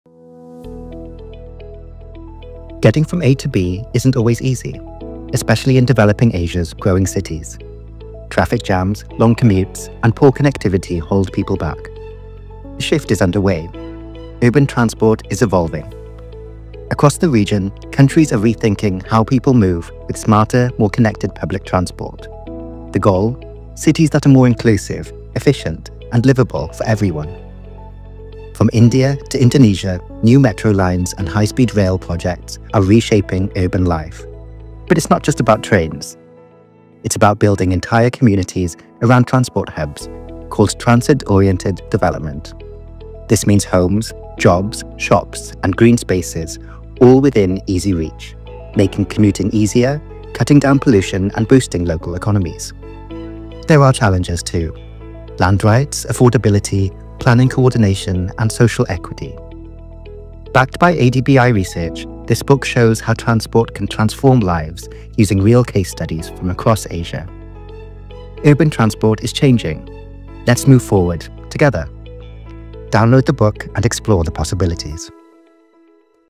Explainer: Urban Transport Transitions in Developing Asia
🎬 Asian cities are taking steps to become more livable and equitable through integrated, accessible, and efficient urban transport systems.